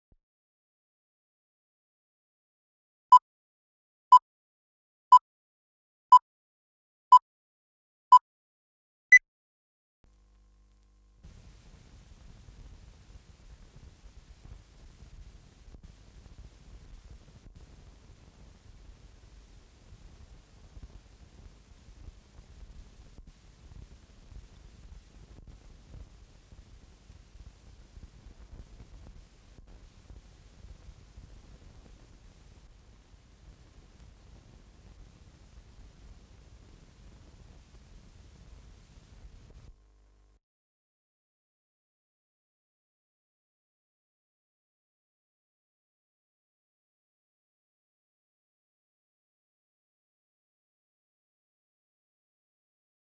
DiFilm - Embajador del Brasil condecora a oficiales de la Armada 1980
Bs. As.: El embajador del Brasil ante el gobierno argentino condecora a tres oficiales de la Armada Argentina. Vistas generales de la ceremonia desarrollada en un salón de la embajada. Vistas generales del público presente. (Color/Sin sonido)
Fecha: 15/12/1980
Duración: 29 segundos
Código del film: A-05207

Buenos Aires: The Ambassador of Brazil to the Argentine government honors three officers of the Argentina Navy. General views of the ceremony developed in the embassy lounge. General views of the public present.